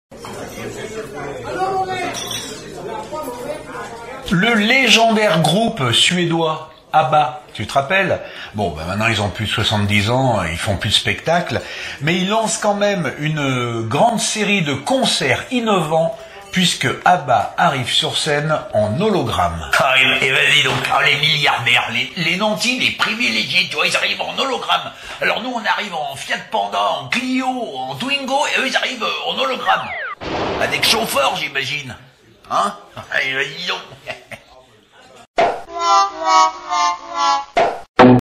Au Bistro - Le groupe Abba revient en concert en hologramme
La célèbre groupe et mythique groupe suedois  ABBA arrive sur scène en hologramme.
Ca fait causer dans les bistros français.

Merci d'avance  ;-)